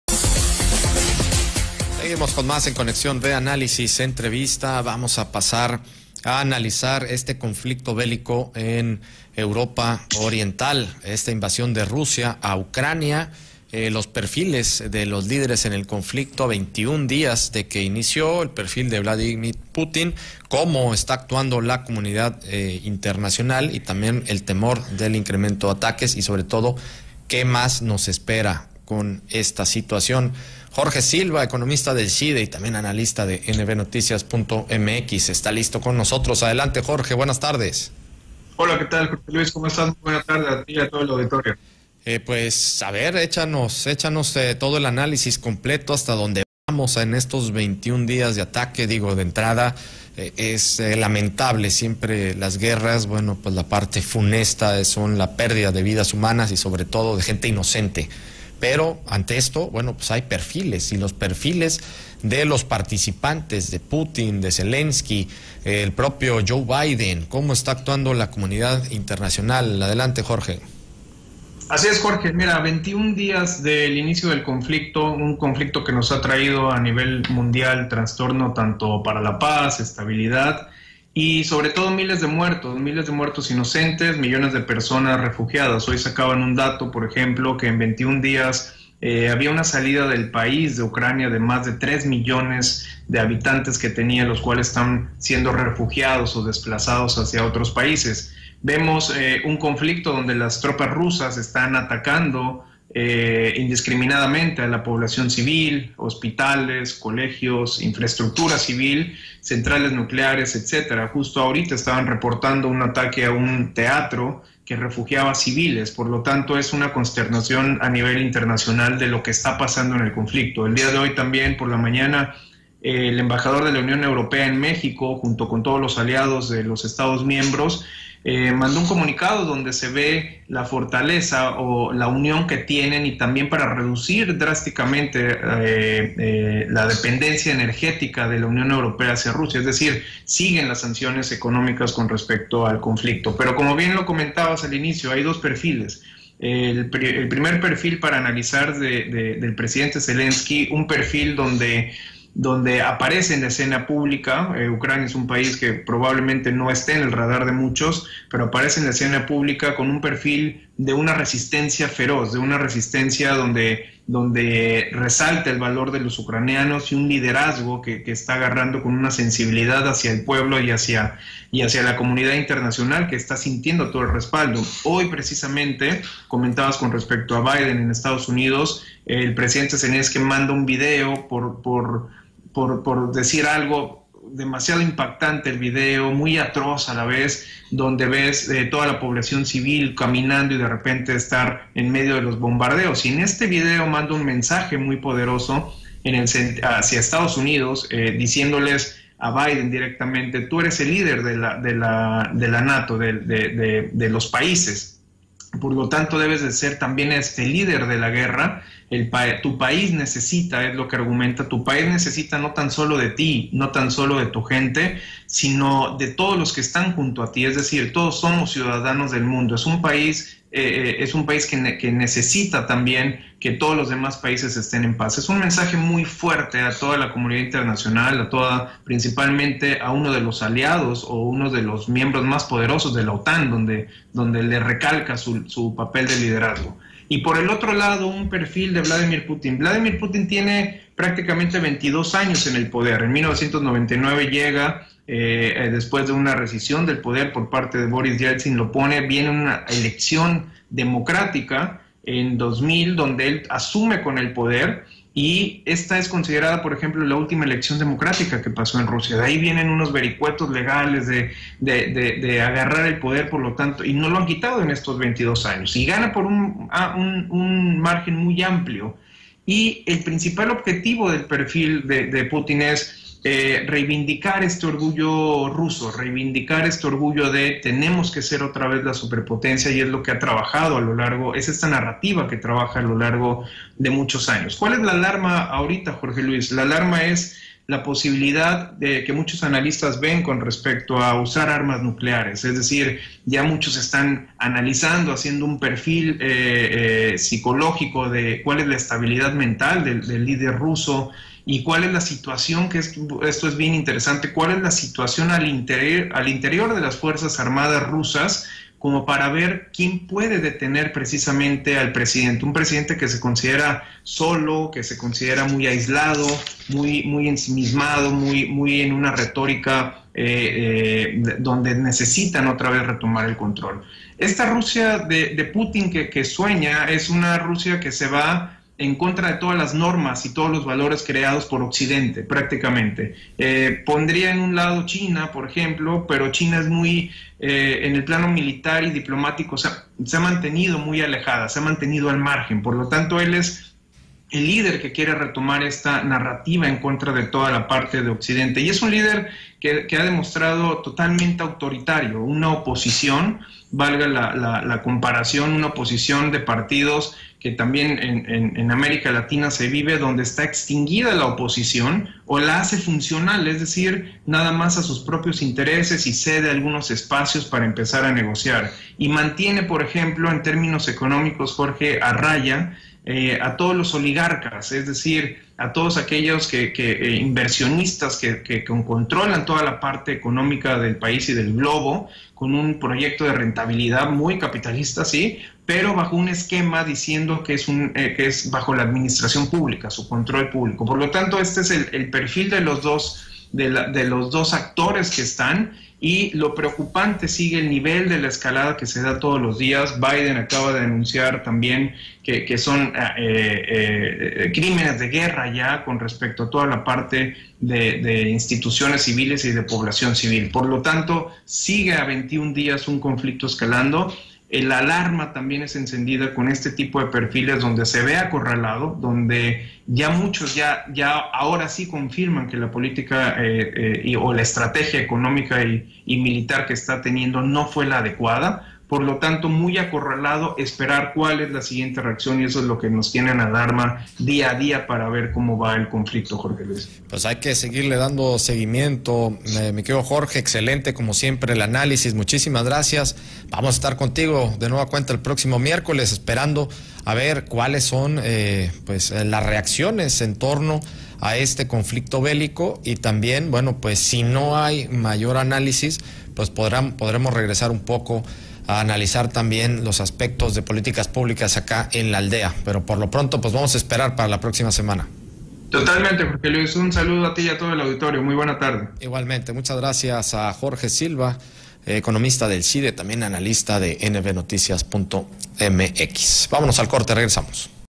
ConexióNV | Perfiles de los líderes en conflicto - Jorge Silva
El académico y economista, Jorge Silva, nos platica sobre los perfiles de los líderes en conflicto.